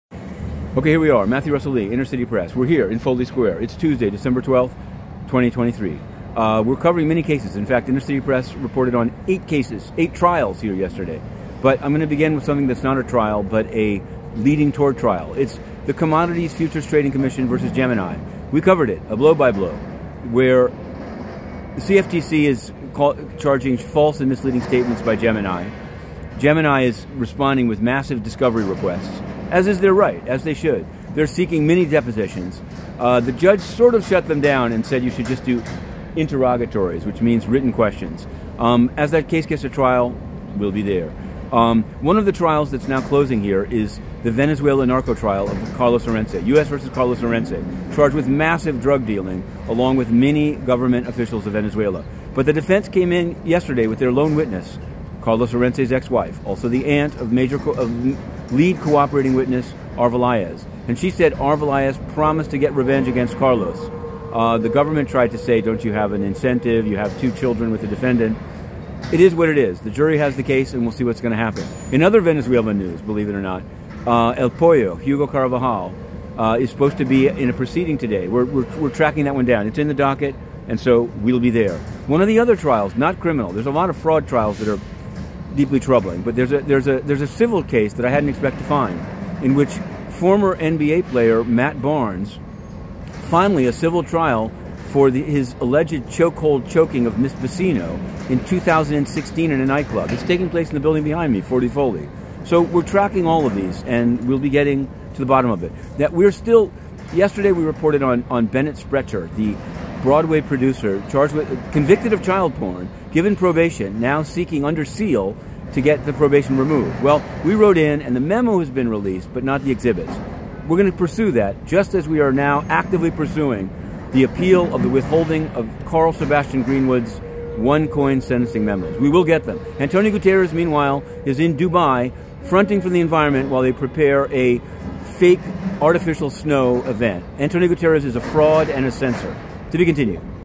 CFTC v. Gemini, VZ Orense jury. xNBA Barnes chokehold trial, Greenwood appeal. UN Guterres fake snow
VLOG Dec 12: CFTC v. Gemini, narcos Venezuela with Orense deliberations and Pollo conference. Ex-NBA Matt Barnes chokehold trial, Sprecher unsealed, Greenwood secrecy appeal. UN @AntonioGuterres at COP28 with ADNOC and artificial snow, fraud & censor